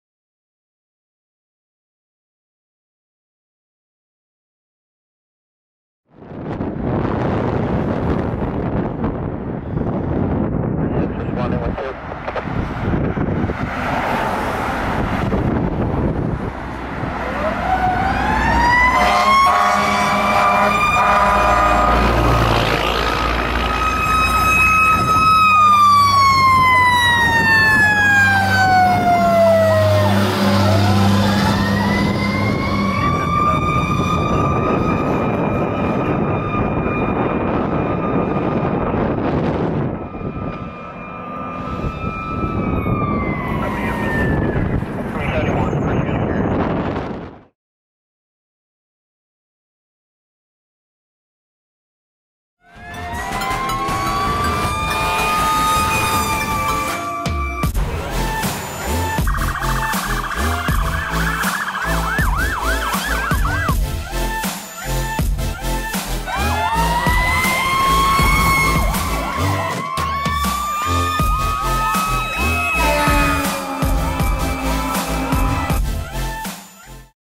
Rocky Run Fire Company Truck 46 Responding 3/8/23
Rocky Run Fire Company Truck 46 Responding in Middletown Township to a building fire in Concord Township, Delaware County PA.
Date: 3/8/23